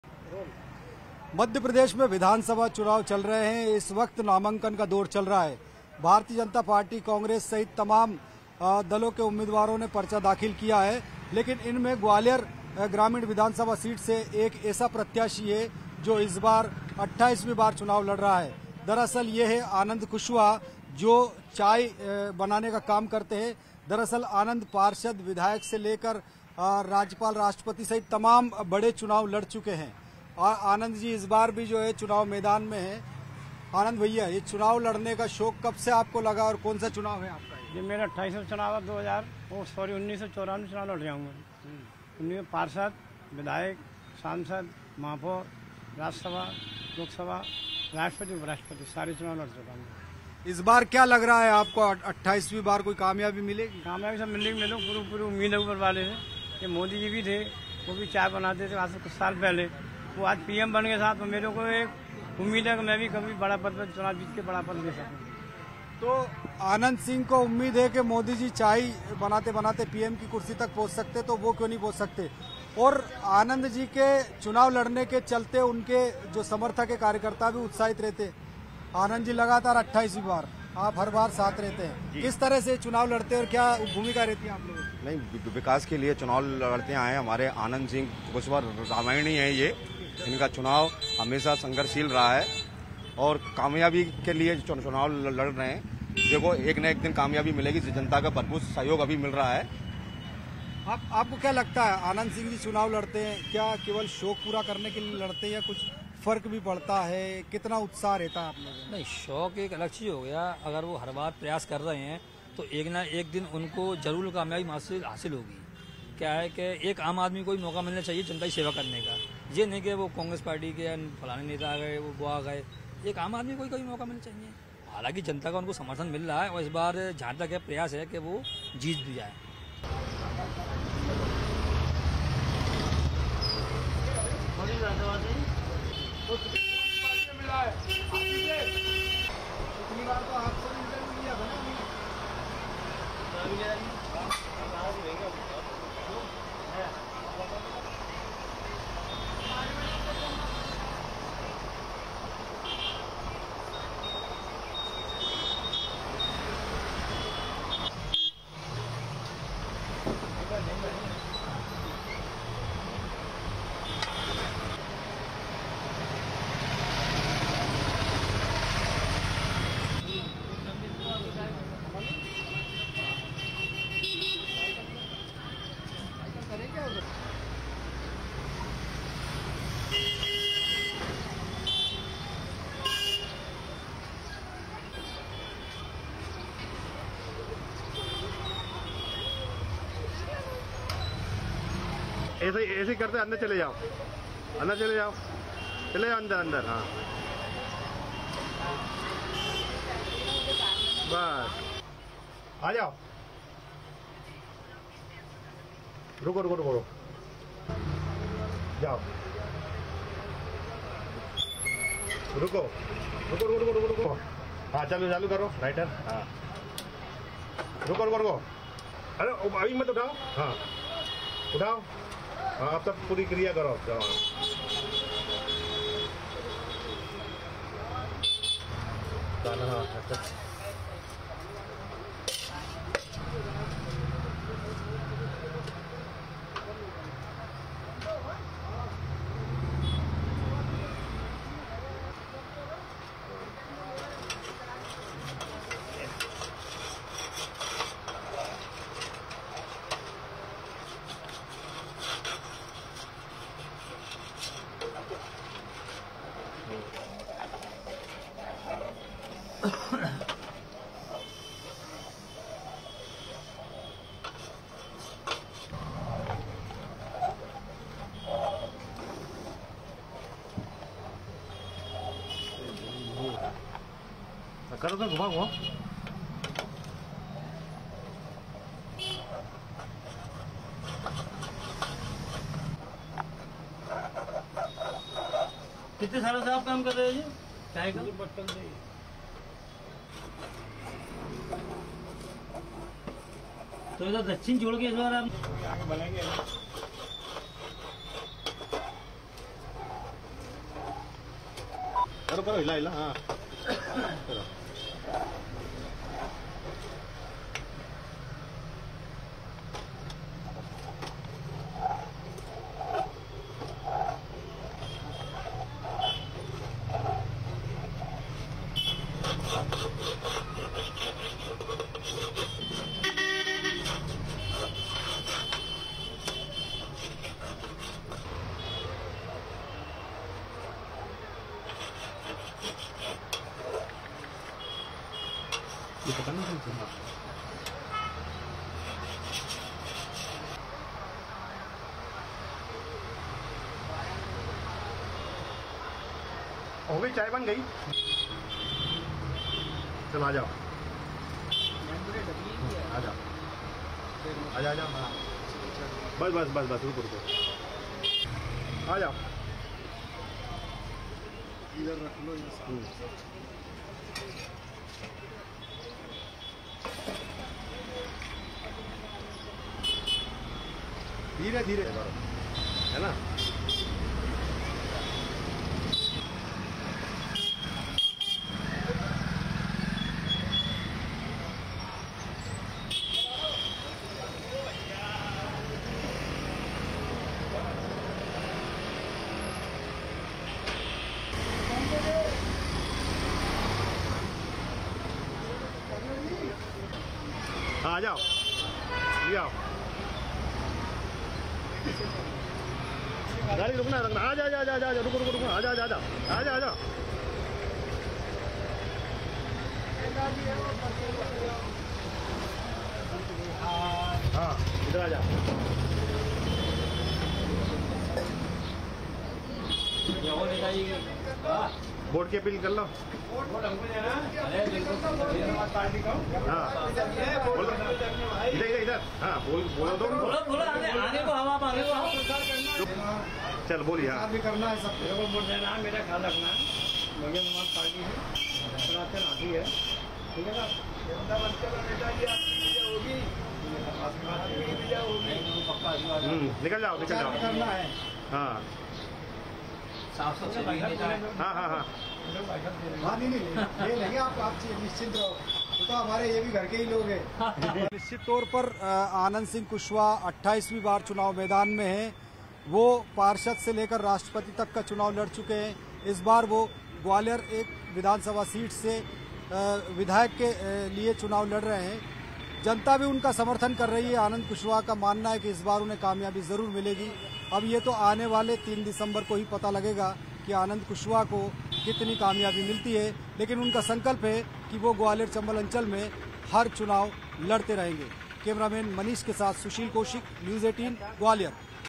MP Assembly Election : पार्षद से लेकर राष्ट्रपति तक 27 बार चुनाव लड़ चुका है Gwalior का यह Chaiwala
MP Assembly Election : पार्षद से लेकर राष्ट्रपति तक 27 बार चुनाव लड़ चुका है Gwalior का यह Chaiwala Anand Singh Kushwaha

Madhya Pradesh Assembly Election 2023 | Gwalior Chaiwala Anand Singh Kushwaha | Gwalior Chaiwala Candidate in MP Election : मध्यप्रदेश में विधानसभा चुनाव की बयार है. इसमें कई दलों के नेता टिकट के लिए बागी हो रहे हैं. वहीं एक चाय बेचने वाले के भी विधानसभा चुनाव लड़ने की चर्चा है. ये शख्स अब तक 27 बार चुनाव लड़ चुका है. ये उनका 28वां चुनाव होने वाला है. इस चाय वाले का नाम आनंद सिंह कुशवाहा है. वे अब तक राष्ट्रपति, उपराष्ट्रपति, सांसद, विधायक और पार्षद तक का चुनाव लड़ चुके हैं. इस बार उन्होंने ग्वालियर पूर्व विधानसभा से बहुजन समाजवादी पार्टी के प्रत्याशी के तौर पर नामांकन पर्चा दाखिल किया है. आनंद सिंह का कहना है कि जब इस देश में चाय बेचने वाला प्रधानमंत्री बन सकता है, तो मैं सांसद, विधायक क्यों नहीं बन सकता हूं. आनंद कुशवाहा इस बार विधानसभा चुनाव का नामांकन दाखिल करने के लिए साइकिल से पहुंचे थे. आनंद कुशवाहा ग्वालियर के रहने वाले हैं. वो एक छोटी सी चाय की दुकान लगाते हैं, लेकिन उनकी राजनीति में गहरी दिलचस्पी है. वे हर चुनाव में नामांकन करते हैं, उन्हें उम्मीद है कि एक ना एक बार जरूर उन्हें जीत मिलेगी.